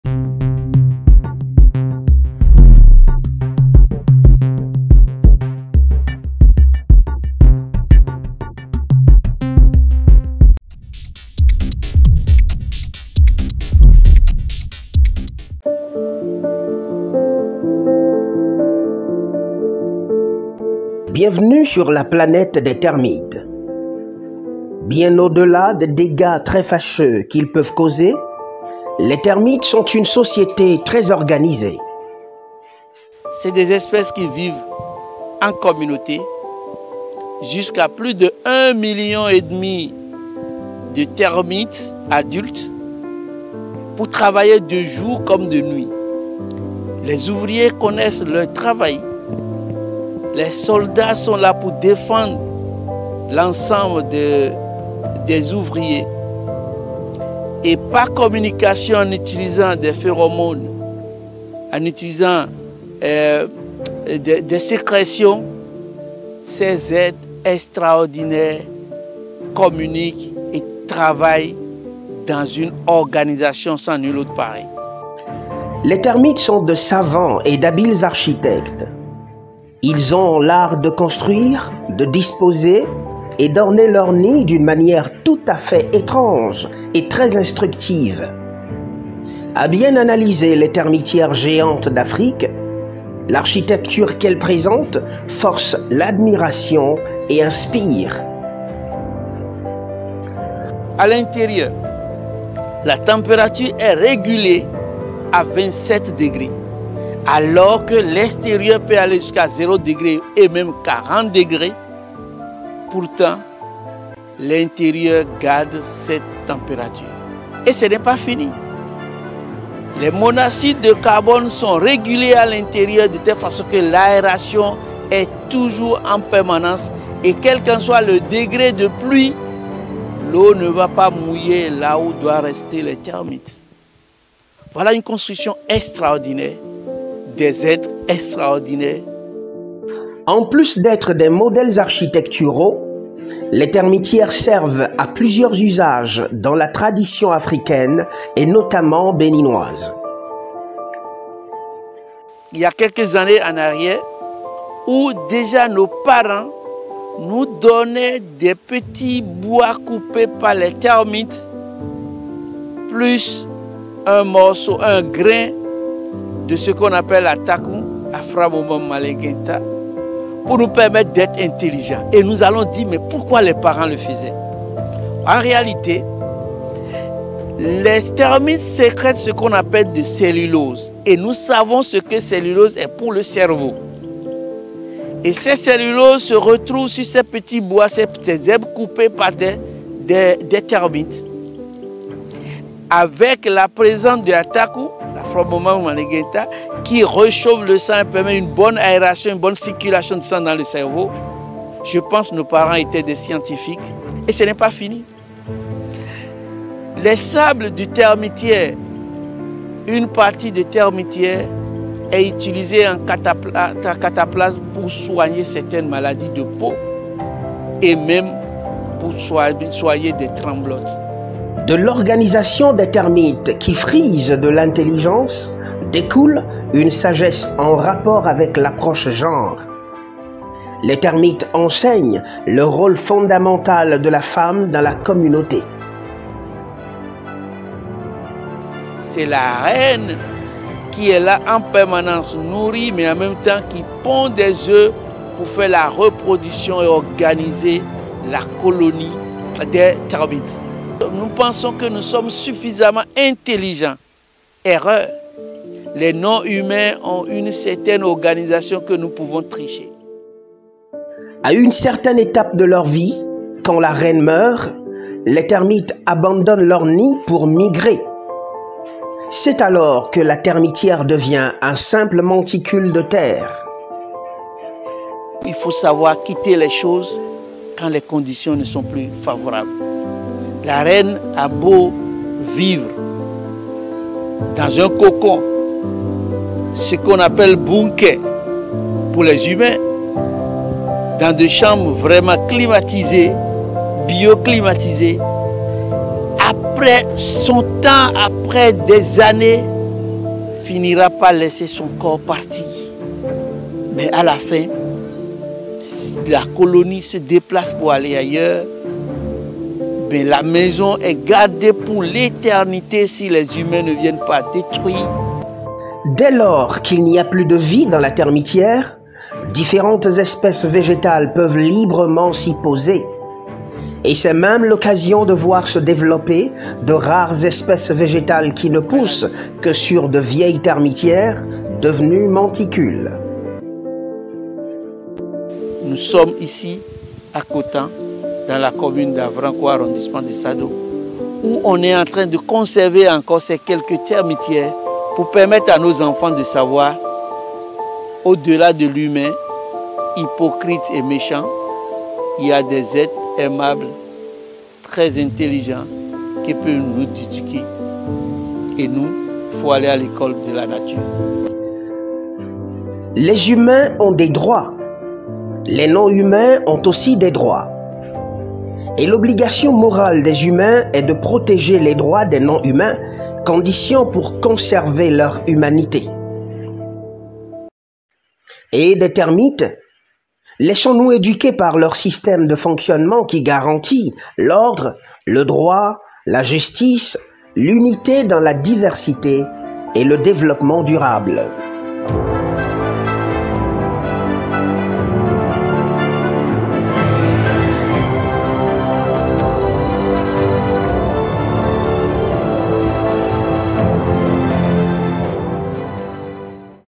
La termitière: œuvre artistique des termites, pleine de sagesse!
Encore appelées fourmis blanches, les termites sont de vraies architectes. A travers la construction des termitières, elles enseignent la sagesse aux êtres humains...